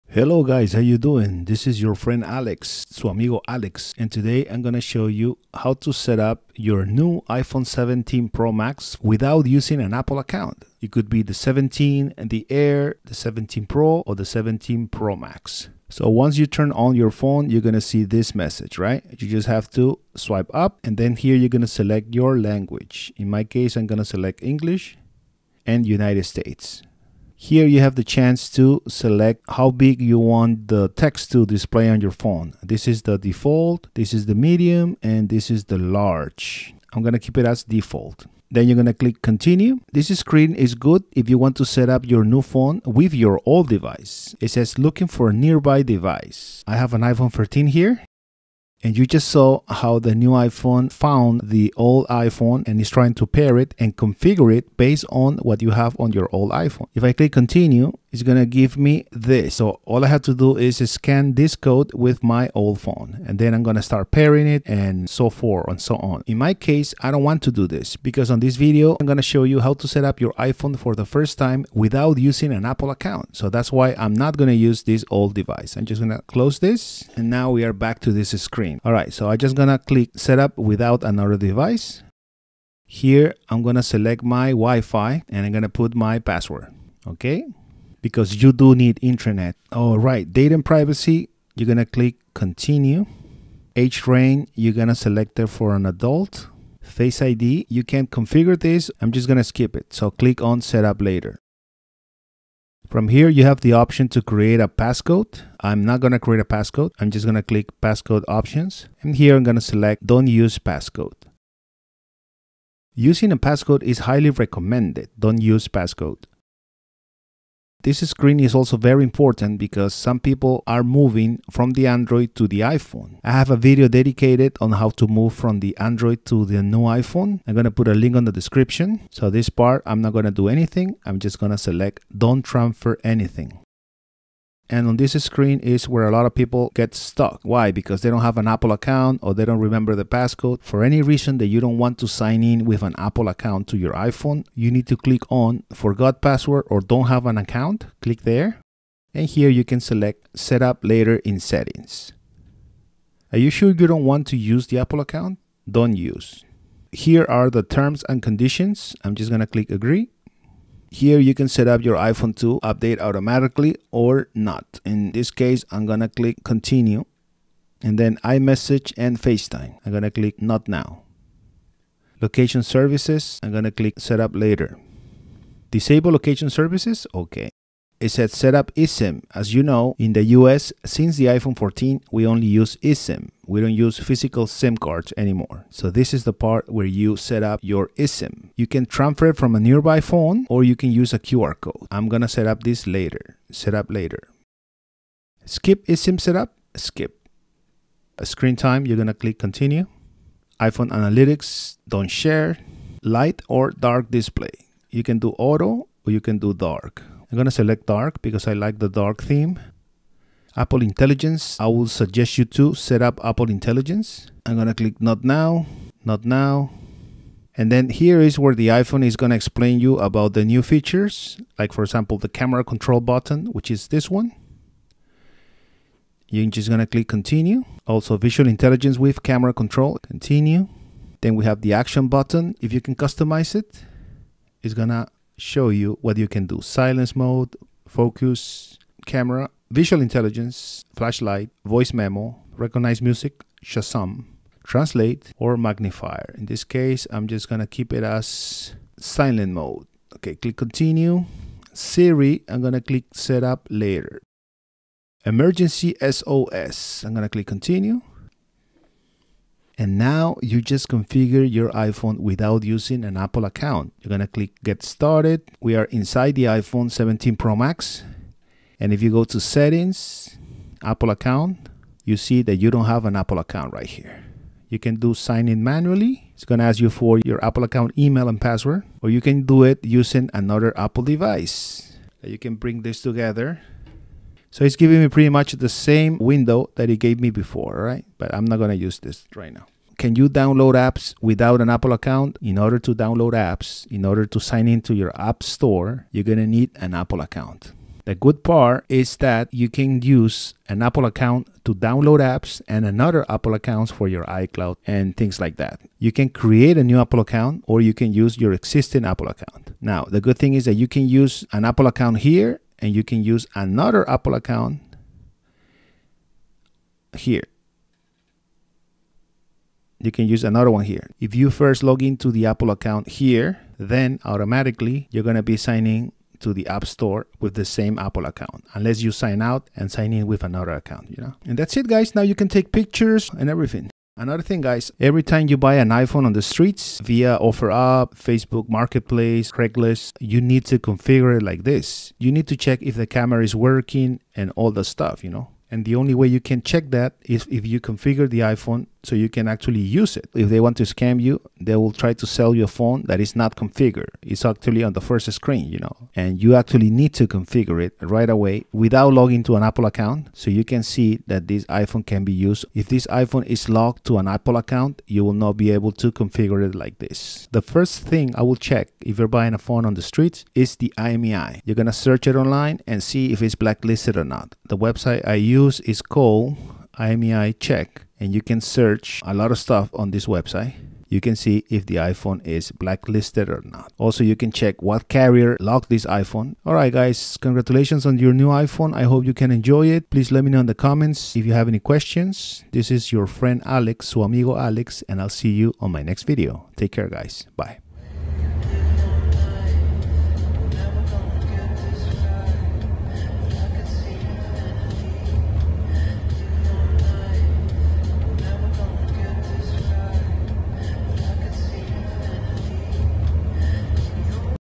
How to Setup your NEW iPhone 17 Pro Max without using an Apple ID Apple Account or iCloud
Video tutorial on how to first setup your new iPhone 17, iPhone 17 Pro, iPhone Air or iPhone 17 Pro Max without using an Apple ID, Apple Account or signing in to your iCloud account.